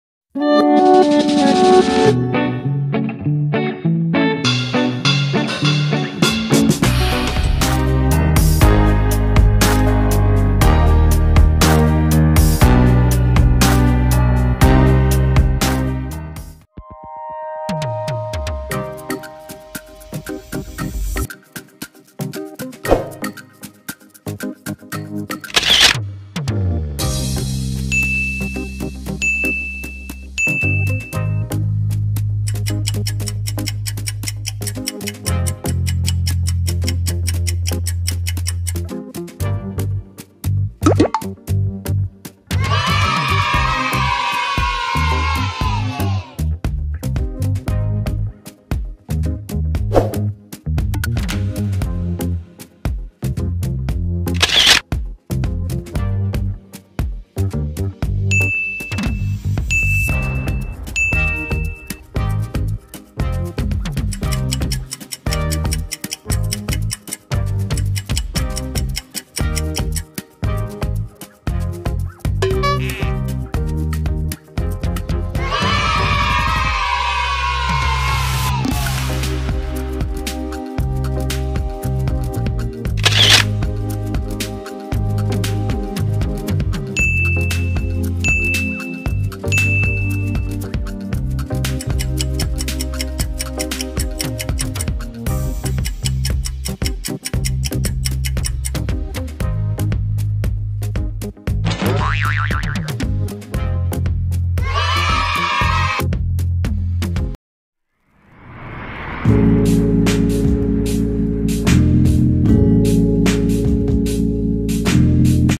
GUESS THE CAR BRAND BY CARS CHARACTER – CAR LOGO QUIZ – Quiz Cars Character Part3 | Noob iQ
Welcome to Noob iQ
In this video, I use the concept of guessing which is slightly different from the previous videos which are usually about super cars and sports cars, this time I will present a guess that is no less exciting, friends, try to guess the brand of the car in the Cars movie character. I'll give you 10 seconds to think. if you need more time please pause.
let's start guessing the guesswork!
Enjooooy!